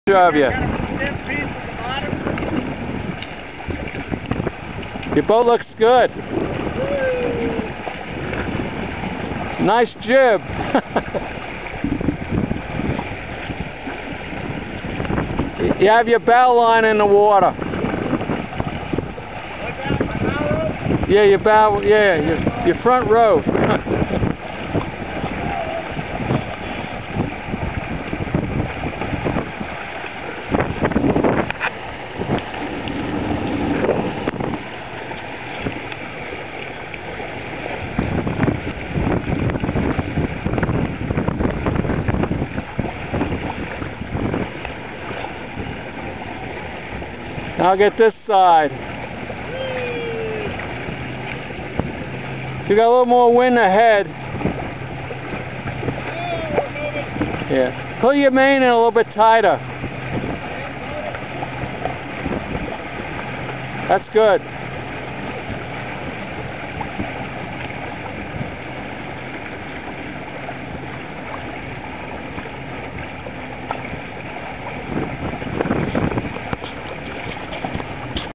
2 Trimaran Sailbirds sailing ; new main 2-10-11 (2).MOV
Darrell with one of my jibs & Ron with the new main  , 2-10-11 wind: 7-10kts